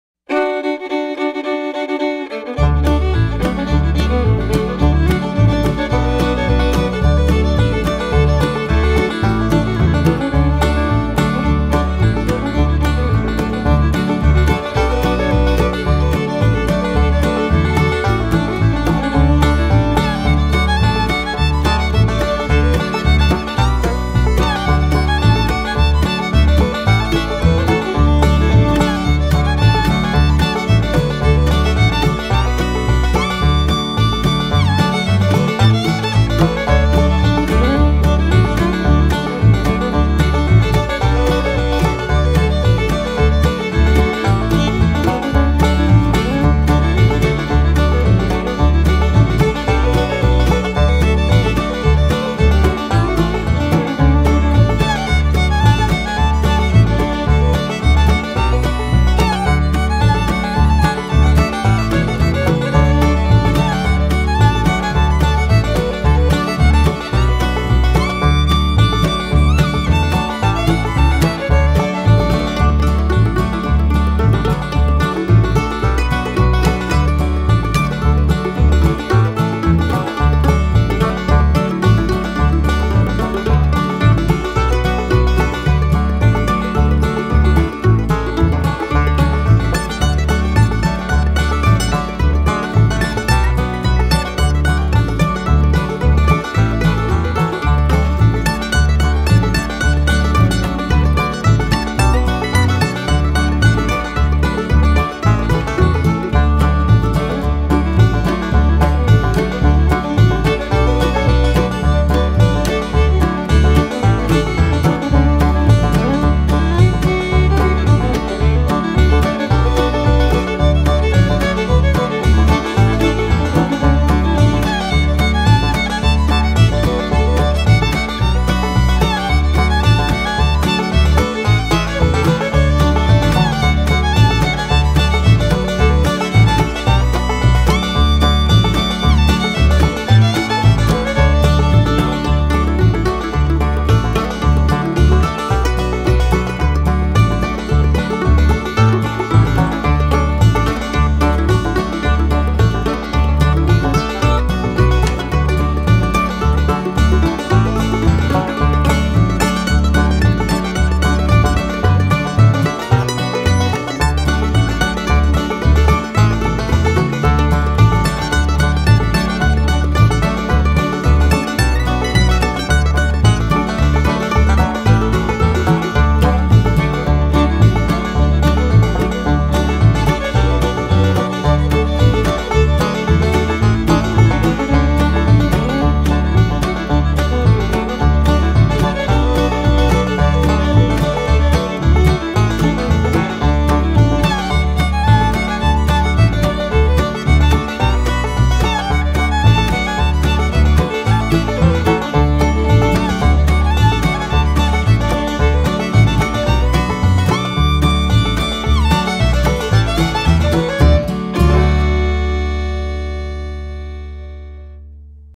Cats and Kittens Playing Fetch
The cats and kittens have discovered a new game of playing fetch on the stairs.